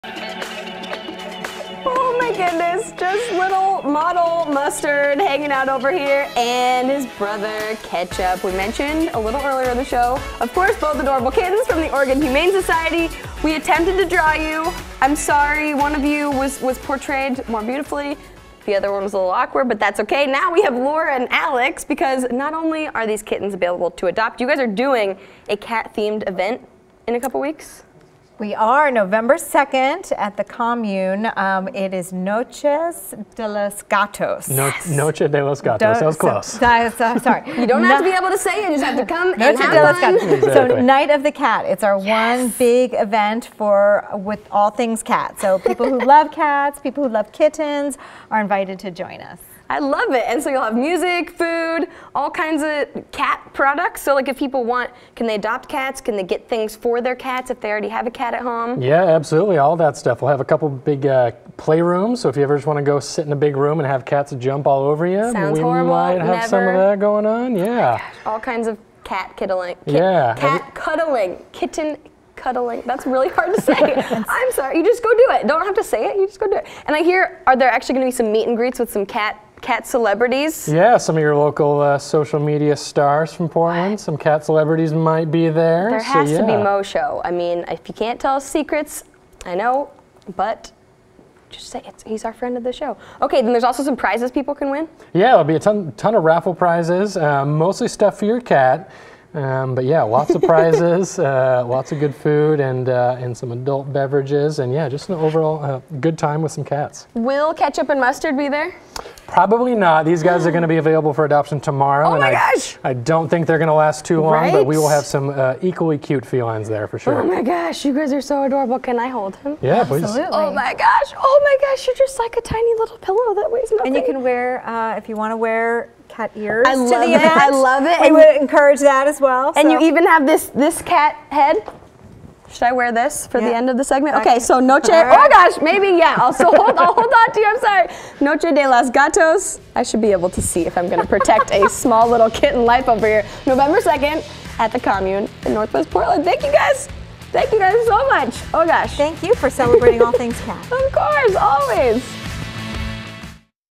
Attend the Oregon Humane Society's cat party
The Oregon Humane Society is throwing a cat-themed party on Nov. 2nd.